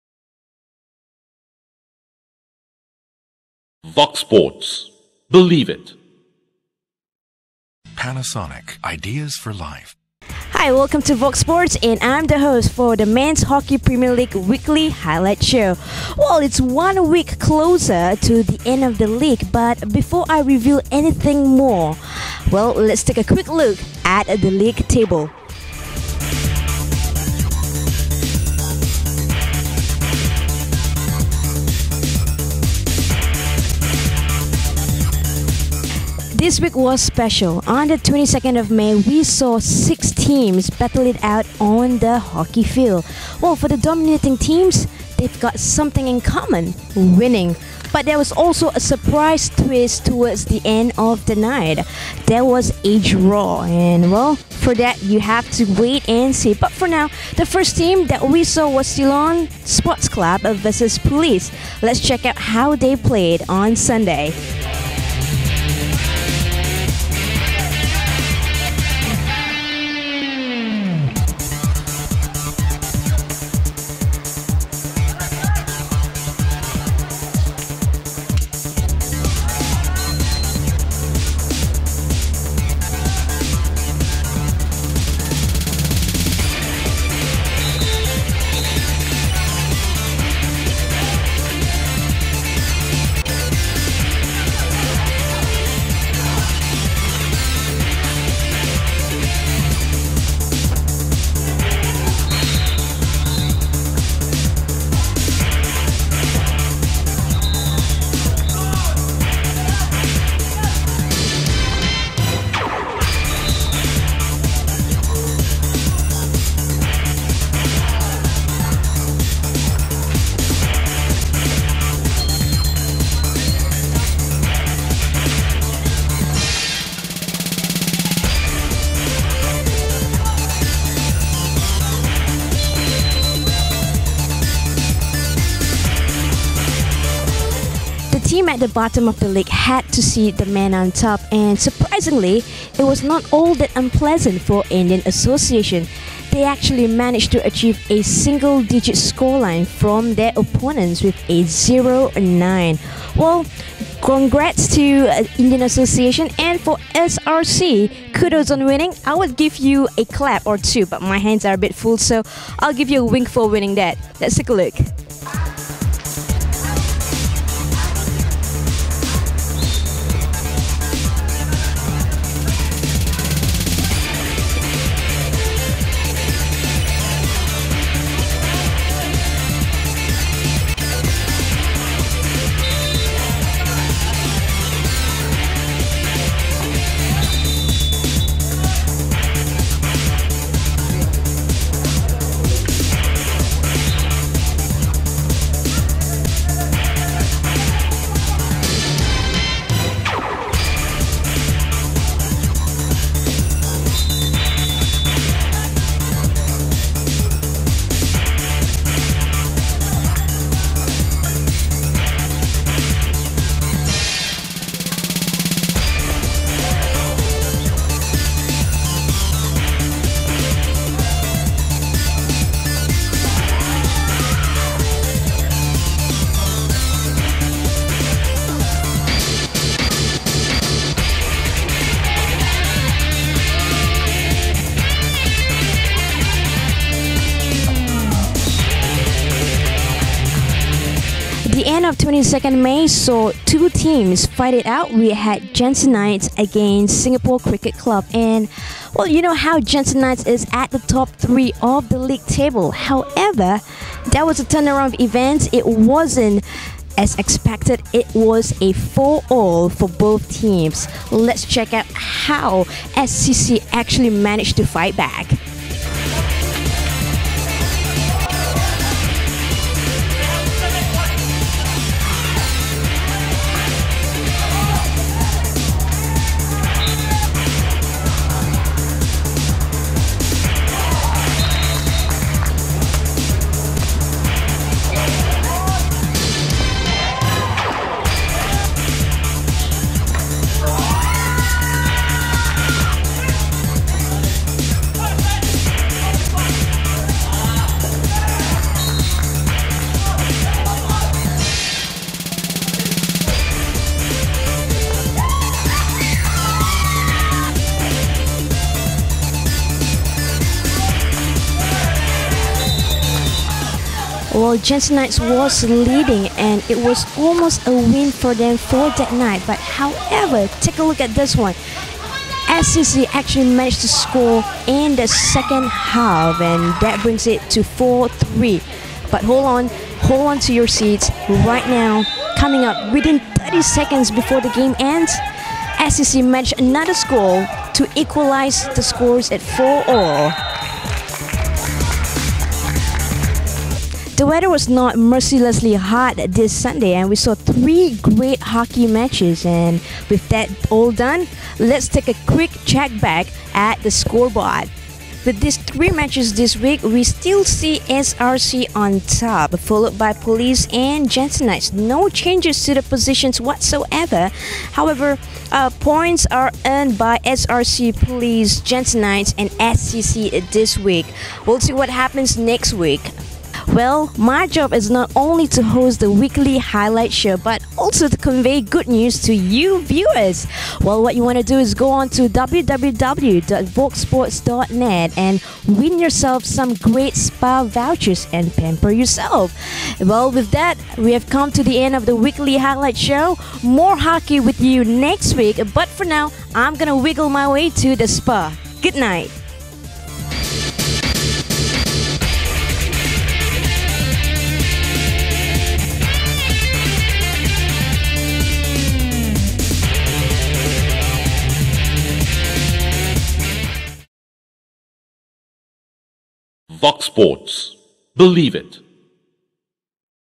Week 9 - Weekly Highlights for Men's Hockey Premier League
22 May 2011

[1] CSC vs Police [8]
[0] IA vs SRC [9]
[4] SCC vs Jansenites [4]